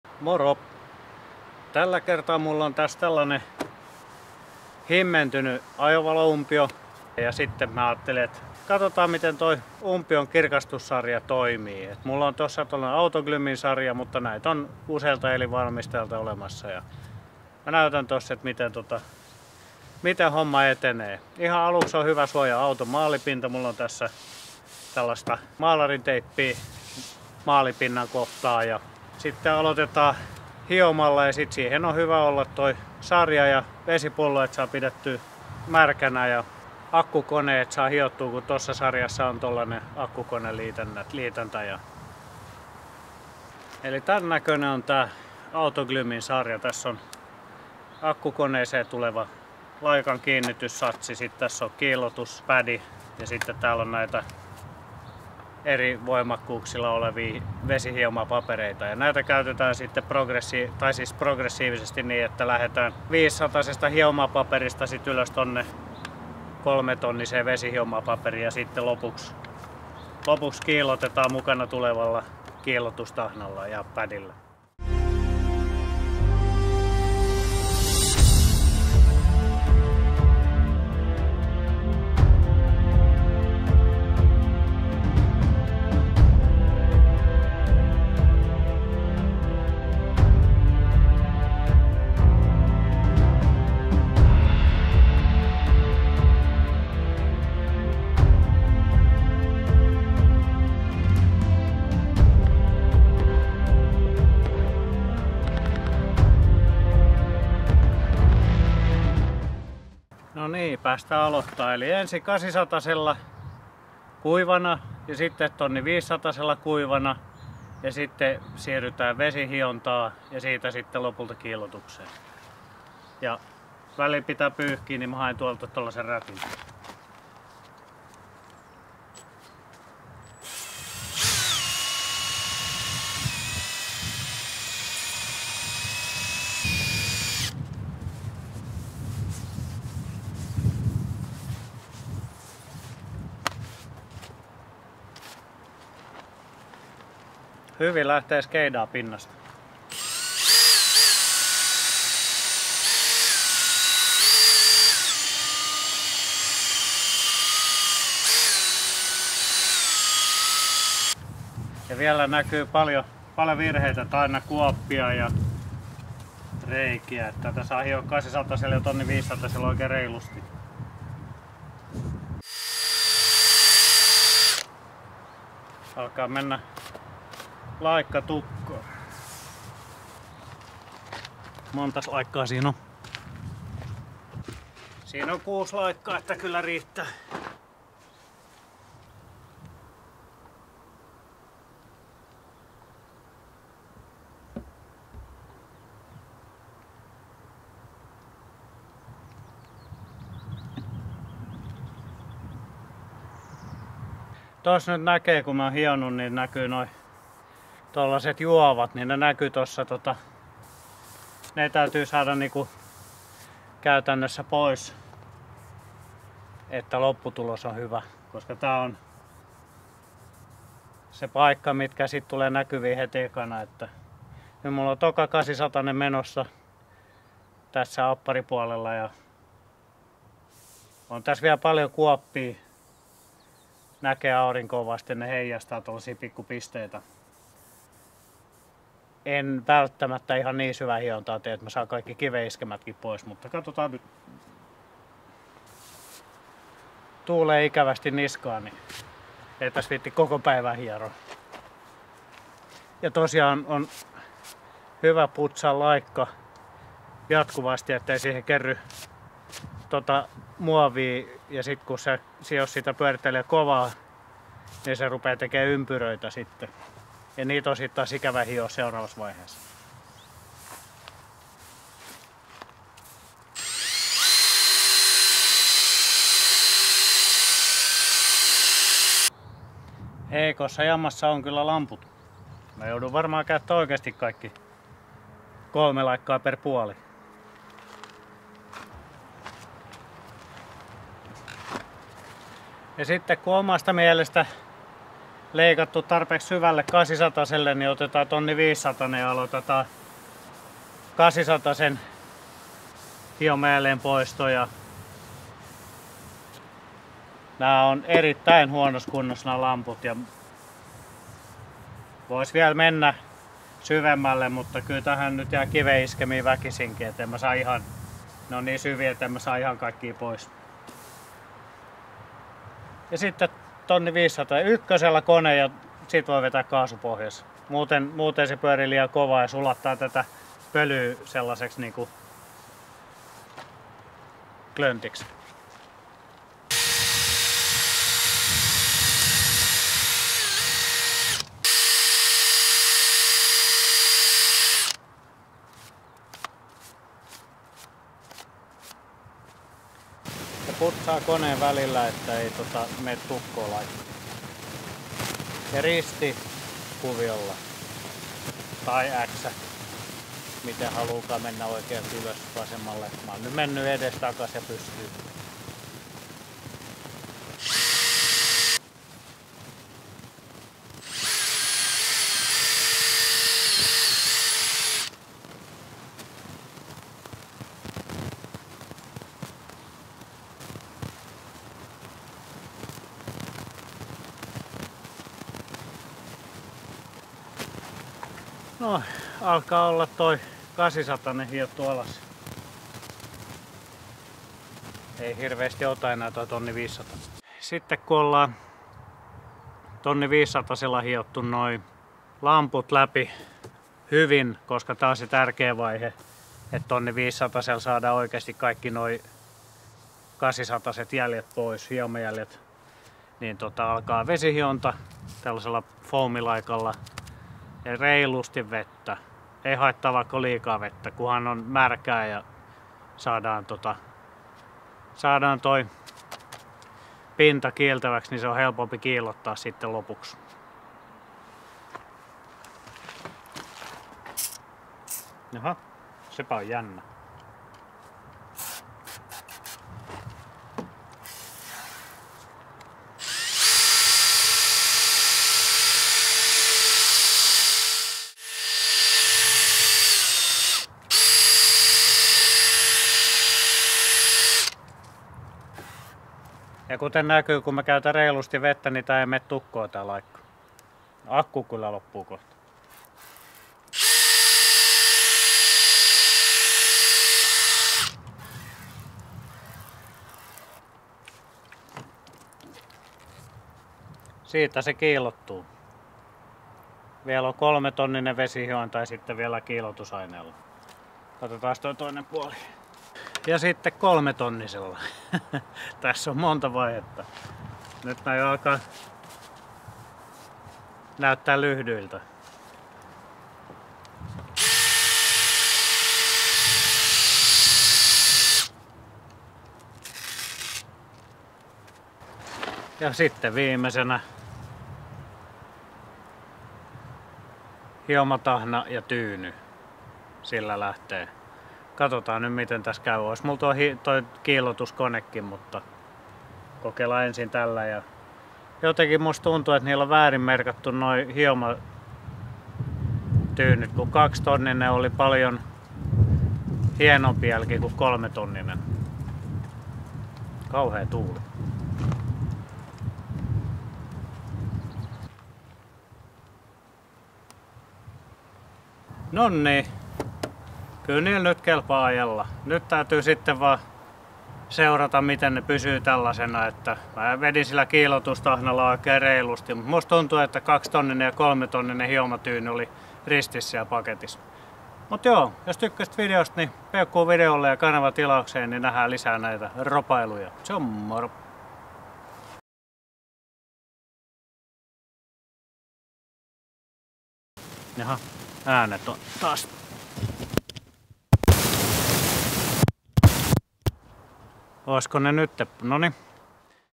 Audin ajovalojen kiillotus
Näytän miten kiillotetaan muoviumpiolla varustetun auton ajovalot. Käytän videolla ostettua kiillotus-sarjaa. Työn voi myös tehdä käsin oikeilla hiomapapereilla ja tahnalla.